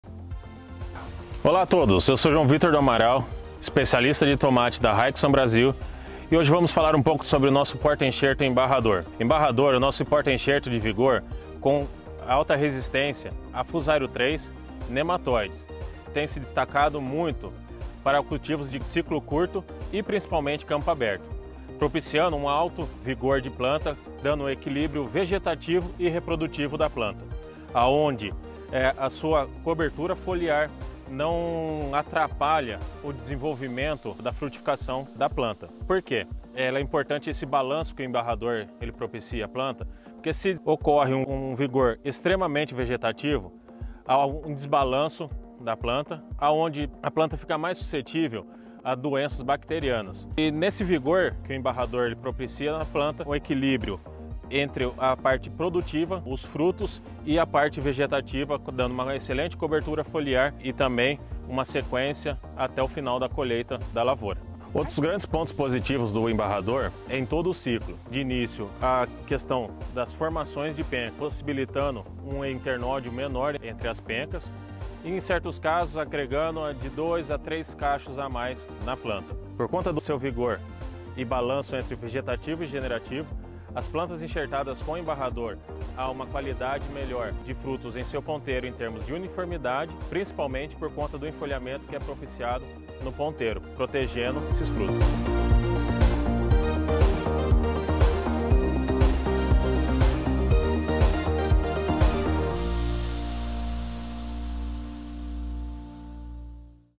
Porta-enxerto para tomate Embajador RZ
Nosso Especialista de Cultivo de Tomate, João Vitor, compartilha conosco sobre nosso Porta-Enxerto para tomates Embajador RZ, ele proporciona alto vigor para a planta, garantindo resistência, equilíbrio vegetativo, reprodutivo e muito mais!
Assista ao vídeo para ver todos os benefícios do Porta-Enxerto Embajador RZ.